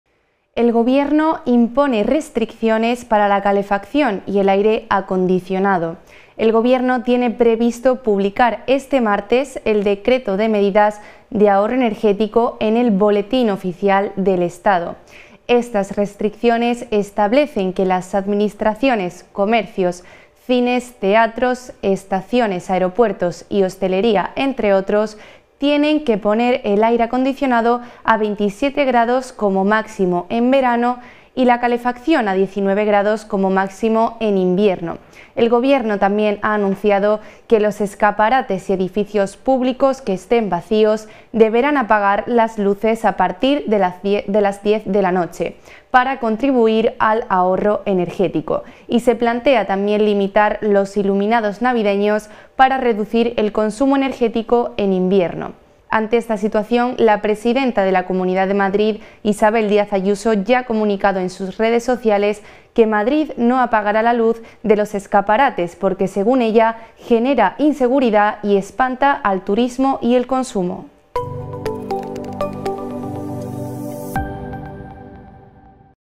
El gobierno impone restricciones para la calefacción y el aire acondicionado
El gobierno tiene previsto publicar este martes el decreto de medidas de ahorro energético en el Boletín Oficial del Estado. Estas restricciones establecen que las administraciones, comercios, cines, teatros, estaciones, aeropuertos y hostelería, entre otros, tienen que poner el aire acondicionado a 27 grados como máximo en verano y la calefacción a 19 grados como máximo en invierno. El gobierno también ha anunciado que los escaparates y edificios públicos que estén vacíos deberán apagar las luces a partir de las diez de la noche para contribuir al ahorro energético, y se plantea limitar los iluminados navideños para reducir el consumo energético en invierno. Ante esta situación, la presidenta de la Comunidad de Madrid, Isabel Díaz Ayuso, ya ha comunicado en sus redes sociales que Madrid no apagará la luz de los escaparates porque, según ella, “genera inseguridad y espanta al turismo y el consumo”.